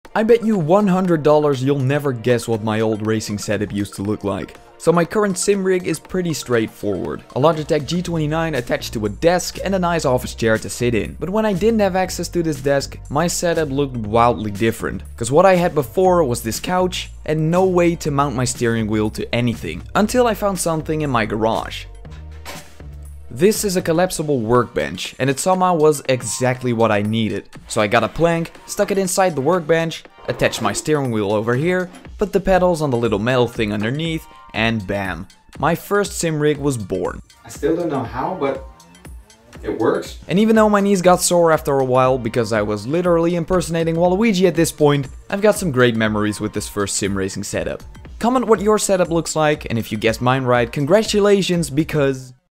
Weirdest "sim rig" I've ever seen
This is what my "sim rig" looked like before I had access to a desk 😂